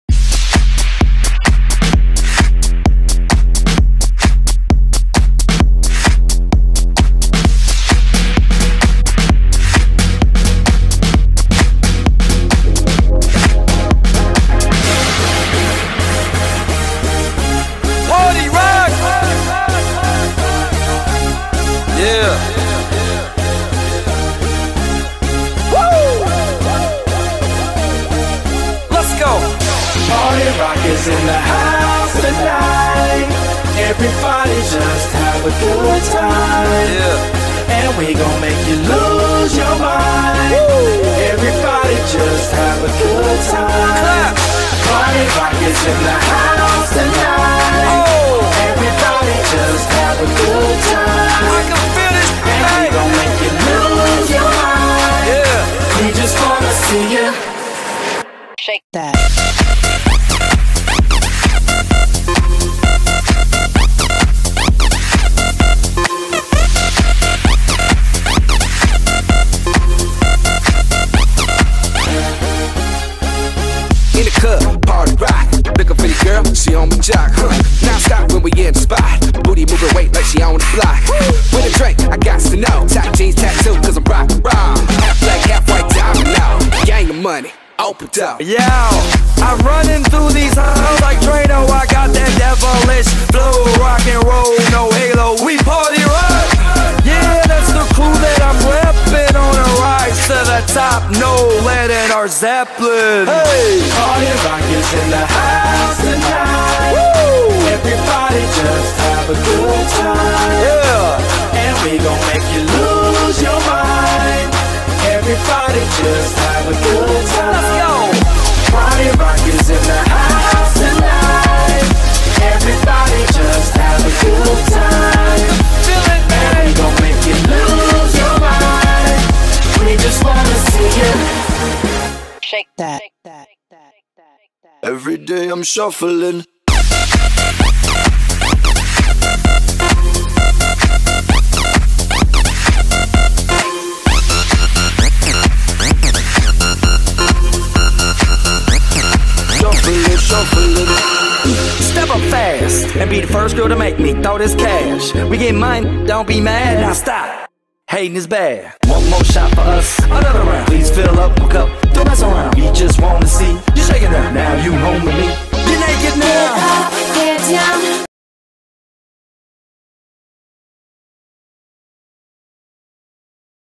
Angry man
Look at the bottom right part of the screen and you'll see a flaming radio